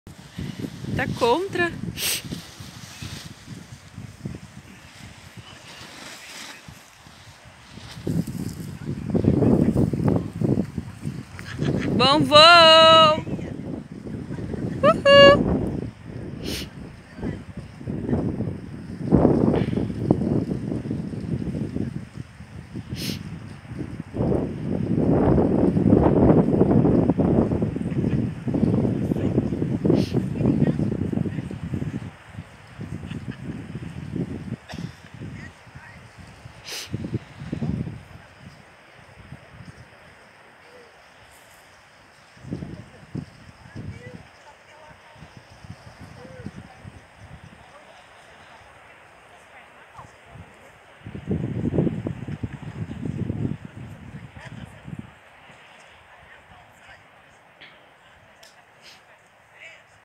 parapente
decolando na rampa de sudeste de baixo praia brava , Florianópolis - SC